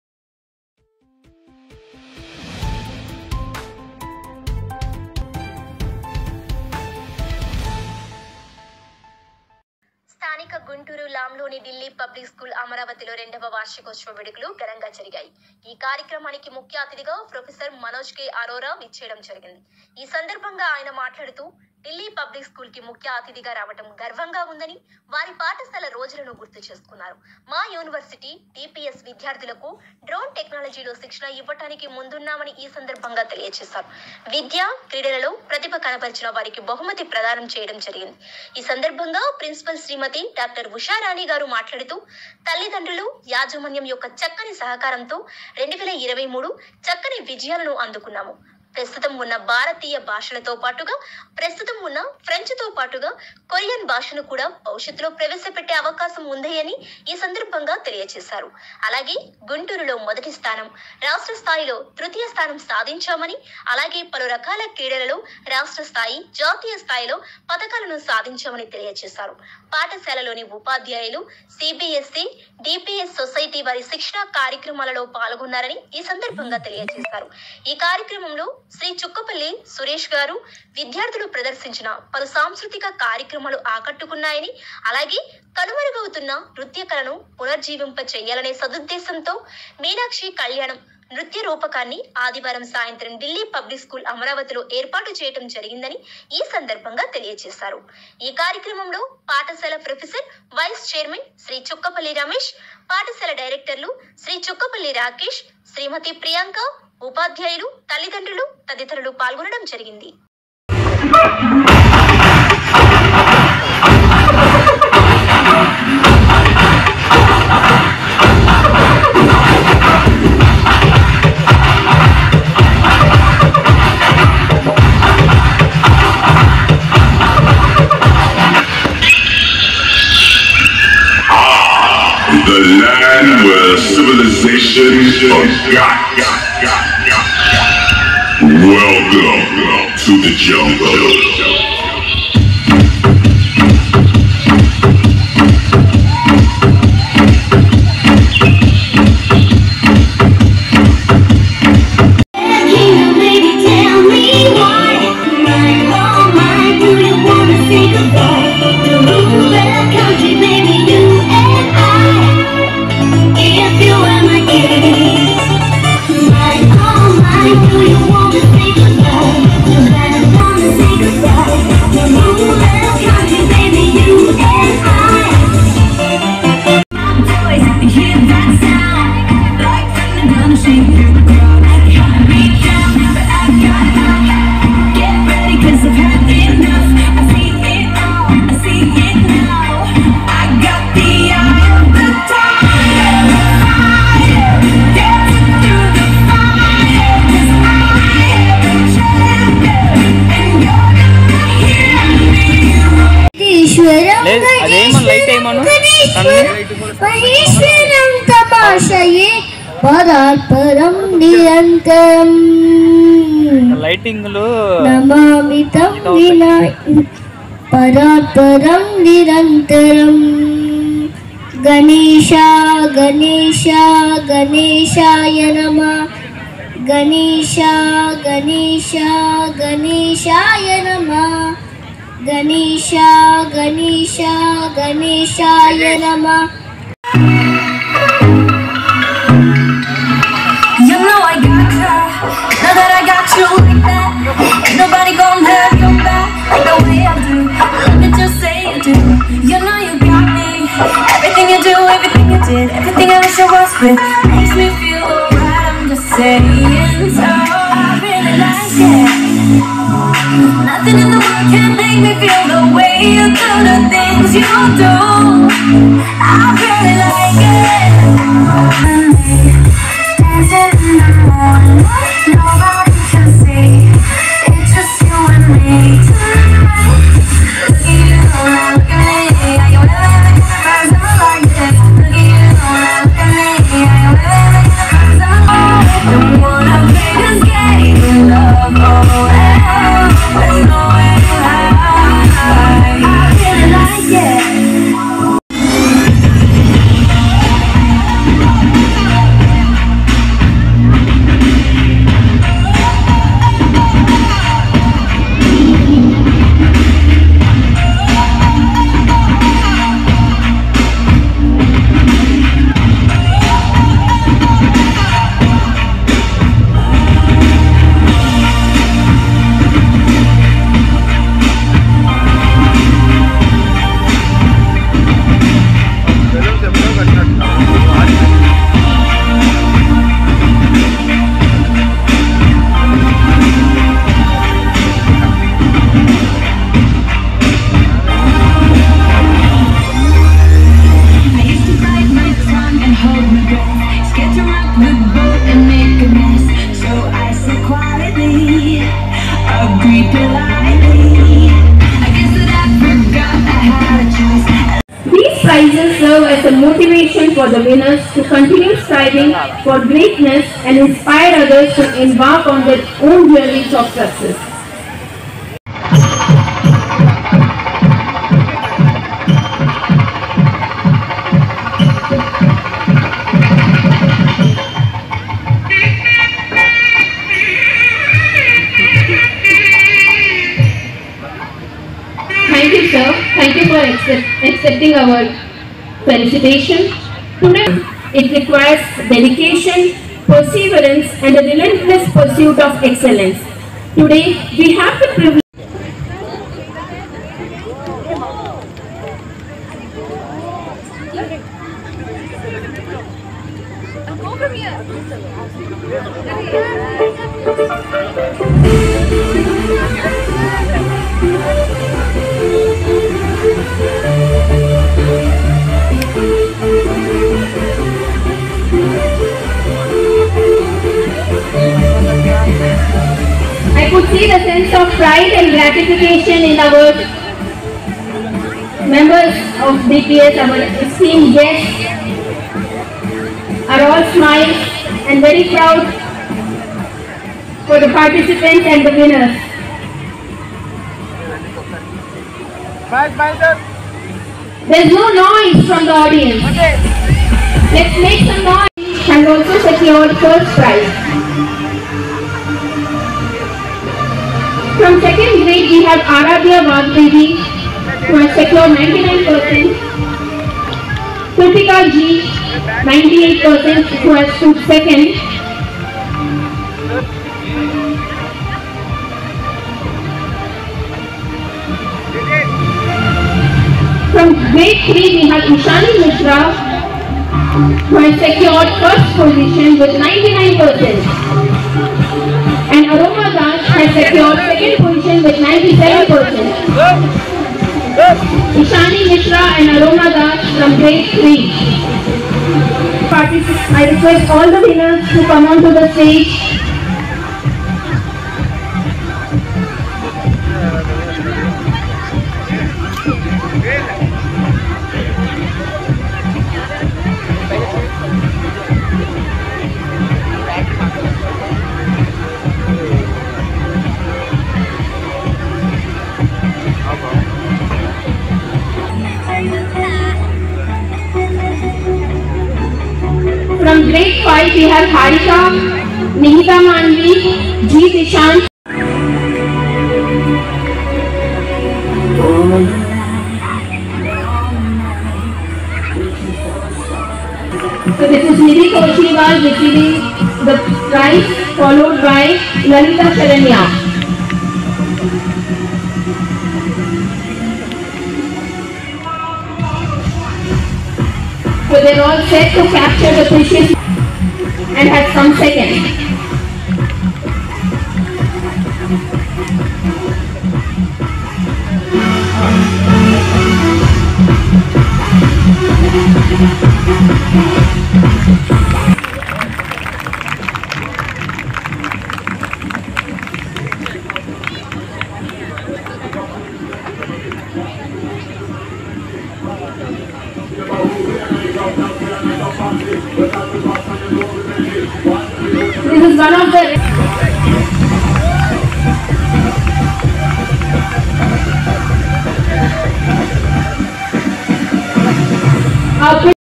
ఢిల్లీ పబ్లిక్ స్కూల్ అమరావతి రెండవ వార్షికోత్సవం
స్థానిక గుంటూరు లాం లోని ఢిల్లీ పబ్లిక్ స్కూల్ అమరావతిలో రెండవ వార్షికోత్సవ వేడుకలు ఘనంగా జరిగాయి. ఈ కార్యక్రమానికి ముఖ్యఅతిథిగా ప్రొఫెసర్ మనోజ్ కే అరోరా( వైస్  ఛాన్సలర్  ఎస్.ఆర్.ఎం యూనివర్శిటీ ఏపీ ) విచ్చేయడం జరిగింది. ఈ సందర్భంగా ఆయన మాట్లాడుతూ ఢిల్లీ పబ్లిక్  స్కూల్ కి  ముఖ్య అతిథిగా రావడం గర్వం ఉందని, వారి పాఠశాల  రోజులను గుర్తు చేసుకున్నారు. మా  యూనివర్సిటీ  డి.పి.ఎస్  విద్యార్థులకు డ్రోన్ టెక్నాలజీలో శిక్షణ ఇవ్వడానికి ముందున్నా మని ఈ సందర్భంగా
తెలియచేశారు . విద్య,క్రీడల్లో ప్రతిభ కనబరిచిన వారికి బహుమతి ప్రధానం చేయడం జరిగింది.
ఈ సంద్భంగా ప్రిన్సిపాల్ శ్రీమతి డాక్టర్ ఉషారాణి గారు మాట్లాడుతూ   తల్లిదండ్రులు,యాజమాన్య యొక్క చక్కని సహకారంతో 2023 చక్కని విజయాలను అందుకున్నాము. ప్రస్తుతం ఉన్న  భారతీయభాషలతో పాటుగా  ప్రస్తుతం ఉన్న ఫ్రెంచ్ తో పాటు కొరియన్ భాషను కూడా భవిష్యత్తులో ప్రవేశపెట్టే అవకాశం ఉందని ఈ సందర్భంగా  తెలియచేశారు. అలాగే గుంటూరు  లో మొదటిస్థానం,రాష్ట్ర స్థాయిలో తృతీయ స్థానం(డే స్కూల్)  సాధించామని, అలాగే పలురకాల క్రీడల్లో రాష్ట్ర స్థాయి, జాతీయ స్థాయిలో పతకాలను సాధించామని తెలియచేశారు. పాఠశాలలోని ఉపాధ్యాయులు
సి.బి.ఎస్.సి, డి.పి. ఎస్ సొసైటీ వారి  శిక్షణా కార్యక్రమాలలో పాల్గొన్నారని ఈ సందర్భంగా తెలియచేశారు.
ఈ కార్యక్రమంలో శ్రీ చుక్కపల్లి సురేష్ గారు (హోనరరీ కౌన్సిల్ జనరల్ ఆఫ్ ద రిపబ్లిక్ ఆఫ్ సౌత్ కొరియా, చైర్మన్ ఎమిరిటస్ ఫినిక్స్ గ్రూప్) విద్యార్థులు ప్రదర్శించిన పలు సాంస్కృతిక కార్యక్రమాలు ఆకట్టుకున్నాయని , అలాగే కనుమరుగవుతున్న నృత్య కళలను పునర్జీవింప చేయాలనే సదుద్దేశంతో (మీనాక్షి కళ్యాణం) నృత్య రూపకాన్ని ఆదివారం సాయంత్రం ఢిల్లీ పబ్లిక్ స్కూల్ అమరావతిలో ఏర్పాటు చేయడం జరిగిందని ఈ సందర్భంగా తెలియజేశారు.
ఈ కార్యక్రమంలో పాఠశాల
ప్రో. వైస్ .చైర్మన్ శ్రీ చుక్కపల్లి రమేష్ గారు, పాఠశాల డైరెక్టర్లు శ్రీ చుక్క పల్లి రాకేష్ గారు, శ్రీమతి ప్రియాంక గారు ఉపాధ్యాయులు, తల్లిదండ్రులు తదితరులు పాల్గొనడం జరిగింది.